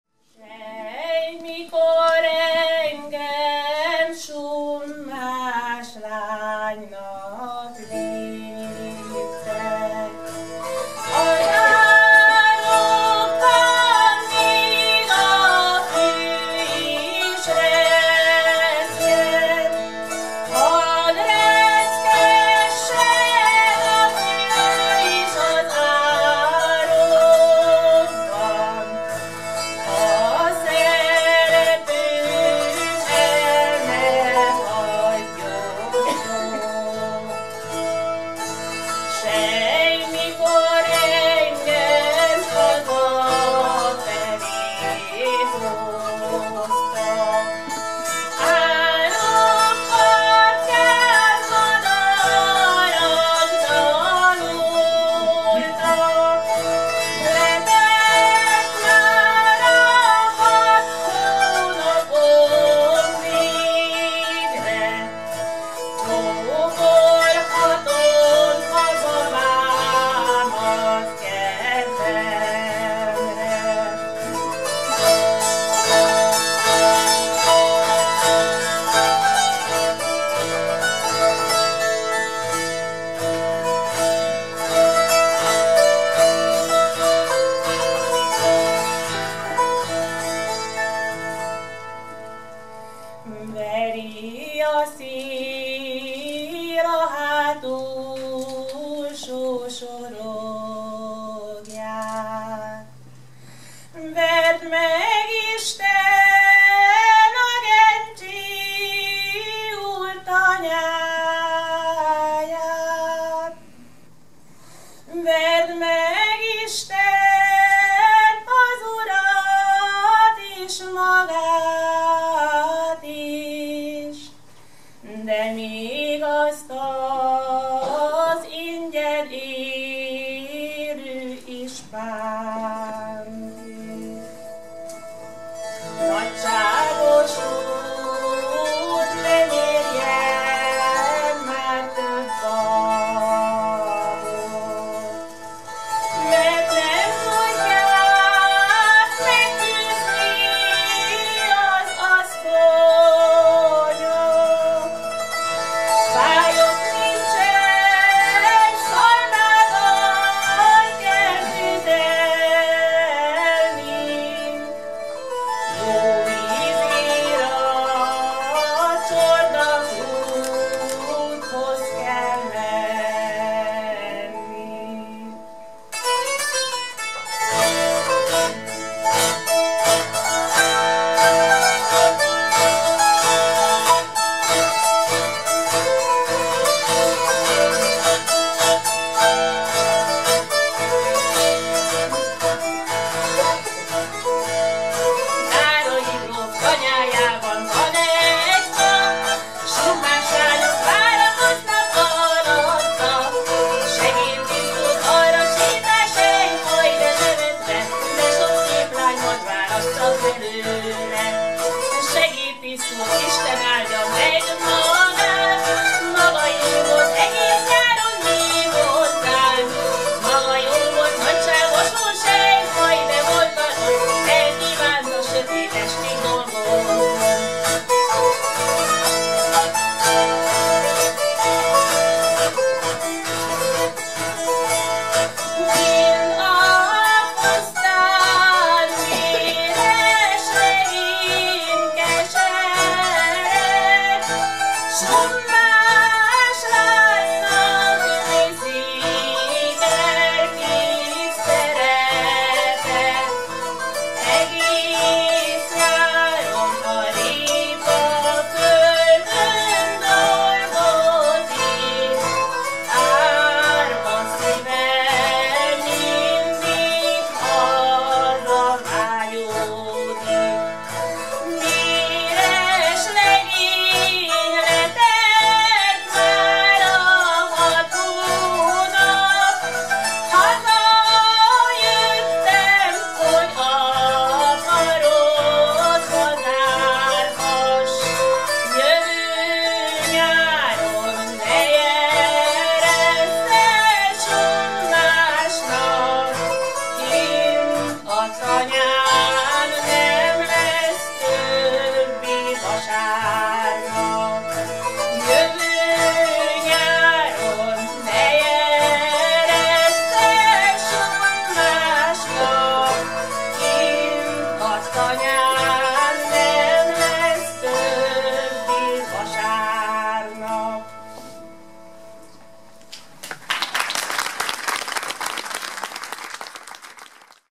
Summásdalok Aradványpusztáról (Oltyán Hermina, Krajcsó Bence)
Summásdalok Aradványpusztáról
Oltyán Hermina (ének), Krajcsó Bence (citera)
Eger, 2019. nov. 17.
Feltöltve az előadók engedélyével.